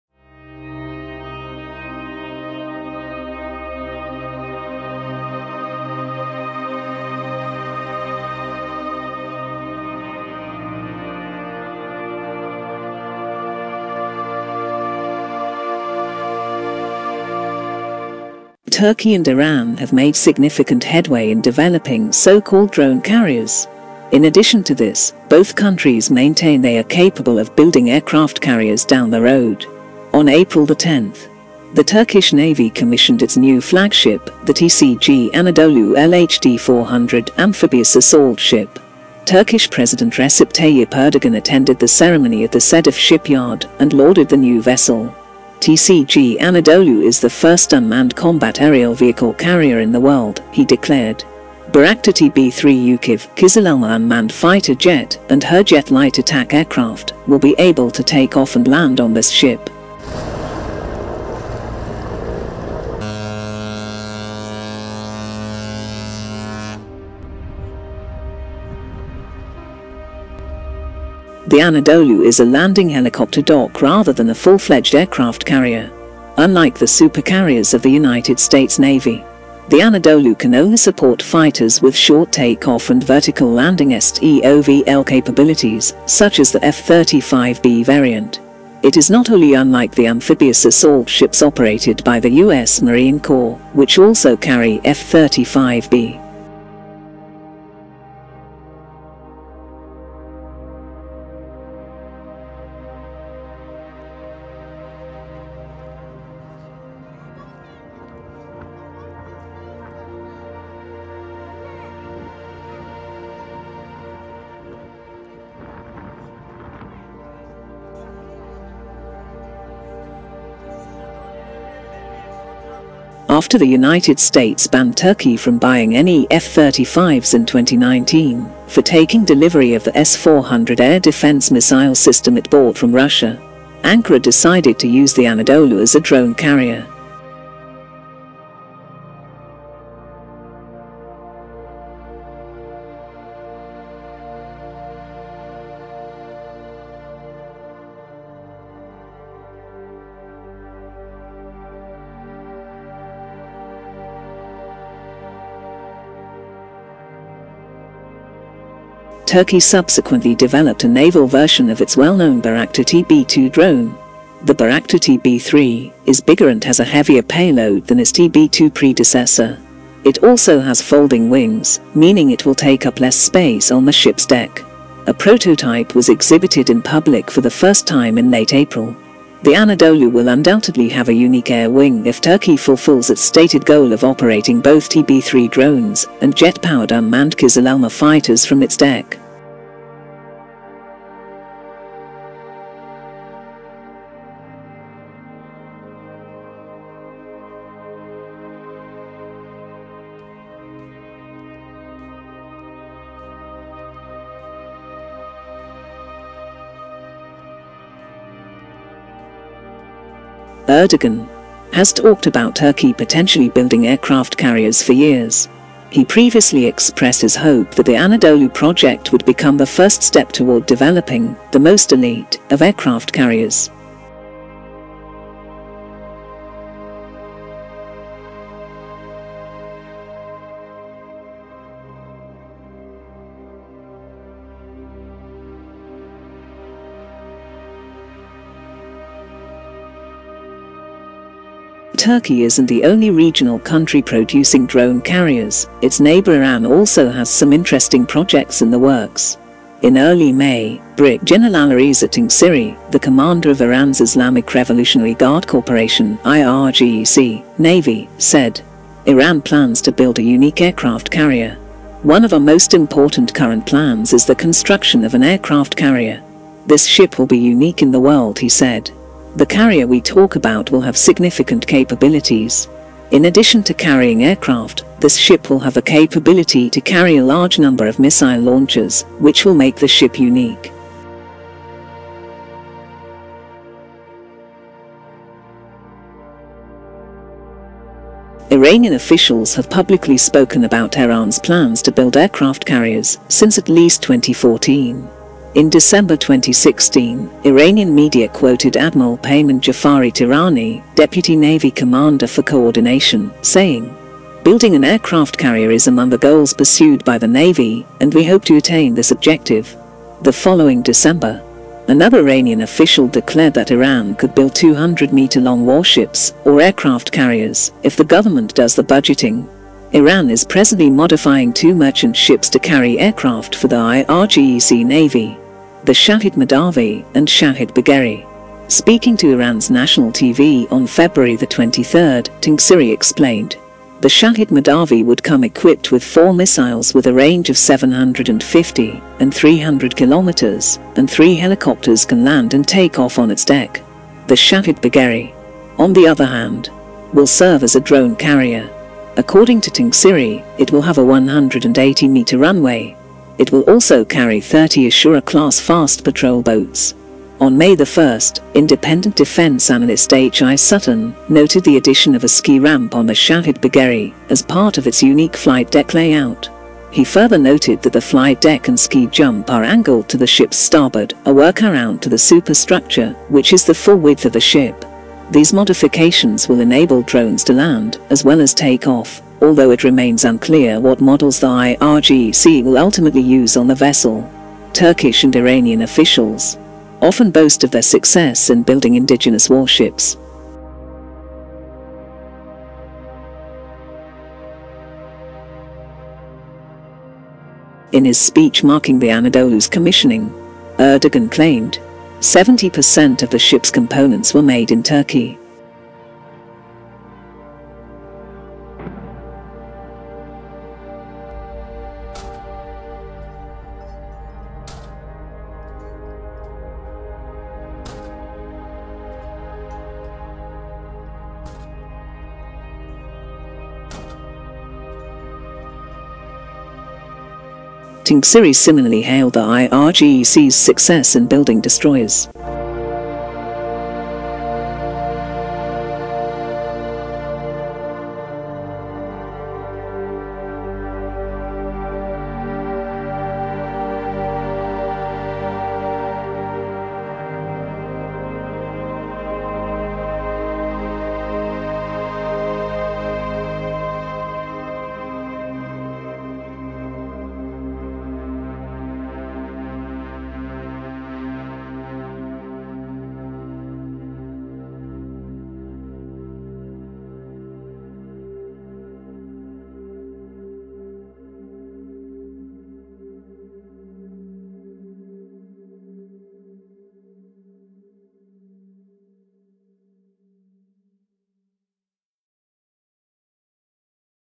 After TCG ANADOLU, Türkiye plans to build the MOST ELITE aircraft carriers, followed by IRAN
Turkey and Iran have made significant headway in developing so-called drone carriers.  In addition to this,  both countries maintain they are capable of building aircraft carriers down the road.

On Apr. 10.  the Turkish Navy commissioned its new flagship, the TCG Anadolu,  (LHD-400) amphibious assault ship.  Turkish President Recep Tayyip Erdogan attended the ceremony at the Sedef Shipyard and lauded the new vessel.

TCG Anadolu is the first unmanned combat aerial vehicle carrier in the world,  he declared.   Bayraktar TB3 UCAV,  Kizilelma unmanned fighter jet,  and Hurjet light attack aircraft,  will be able to take off and land on this ship.

The Anadolu is a landing helicopter dock rather than a full-fledged aircraft carrier.  Unlike the supercarriers of the United States Navy.  the Anadolu can only support fighters with short take-off and vertical landing STOVL capabilities,  such as the F-35B variant.  It is not wholly unlike the amphibious assault ships operated by the U.S. Marine Corps,  which also carry F-35B.

After the United States banned Turkey from buying any F-35s in 2019,  for taking delivery of the S-400 air defense missile system it bought from Russia.   Ankara decided to use the Anadolu as a drone carrier.  Turkey subsequently developed a naval version of its well-known Bayraktar TB2 drone.  The Bayraktar TB3,  is bigger and has a heavier payload than its TB2 predecessor.  It also has folding wings,  meaning it will take up less space on the ship’s deck.  A prototype was exhibited in public for the first time in late April.

The Anadolu will undoubtedly have a unique air wing if Turkey fulfills its stated goal of operating both TB3 drones,  and jet-powered unmanned Kizilelma fighters from its deck.

Erdogan.  has talked about Turkey potentially building aircraft carriers for years.  He previously expressed his hope that the Anadolu project would become the first step toward developing,  “the most elite”,  of aircraft carriers.

Turkey isn’t the only regional country producing drone carriers,  Its neighbor Iran also has some interesting projects in the works.

In early May,  Brig. Gen.  Alireza Tangsiri,  the commander of Iran’s Islamic Revolutionary Guard Corp,  (IRGC),  Navy,  said.  Iran plans to build a “unique” aircraft carrier.

One of our most important current plans is the construction of an aircraft carrier.  This ship will be unique in the world,”  he said.  The carrier we talk about will have significant capabilities.  In addition to carrying aircraft, this ship will have the capability to carry a large number of missile launchers,  which will make the ship unique.

Iranian officials have publicly spoken about Tehran’s plans to build aircraft carriers,  since at least 2014.  In December 2016,  Iranian media quoted Admiral Peiman Jafari Tehrani, Deputy Navy commander for Coordination, saying. Building an aircraft carrier is among the goals pursued by the navy and we hope to attain this objective.

The following December. another Iranian official declared that Iran could build 200-meter-long warships,  or aircraft carriers if the government does the budgeting.

Iran is presently modifying two merchant ships to carry aircraft for the IRGC-Navy.   the Shahid Mahdavi,  and Shahid Bagheri.

Speaking to Iran’s national TV on Feb. 23,  Tangsiri explained.  the Shahid Mahdavi would come equipped with four “missiles with a range of 750,  and 300 km,  and three helicopters can land and take off on its deck.

The Shahid Bagheri.  on the other hand. will serve as a drone carrier.  According to Tangsiri,  it will have a 180-meter runway.  It will also carry 30 Ashura-class fast patrol boats.

On May 1,  independent defense analyst H I Sutton noted the addition of a ski ramp on the Shahid Bagheri as part of its “unique flight deck layout.”

He further noted that the flight deck and ski-jump are angled to the ship’s starboard,  “a workaround to the superstructure which is the full width of the ship.”  These modifications will enable drones “to land as well as take off,”  although it remains unclear what models the IRGC will ultimately use on the vessel.

Turkish and Iranian officials.  often boast of their success in building indigenous warships.  In his speech marking the Anadolu’s commissioning. Erdogan claimed.  70 percent of the ship’s components were made in Turkey.  Tangsiri similarly hailed the IRGC’s success in building destroyers.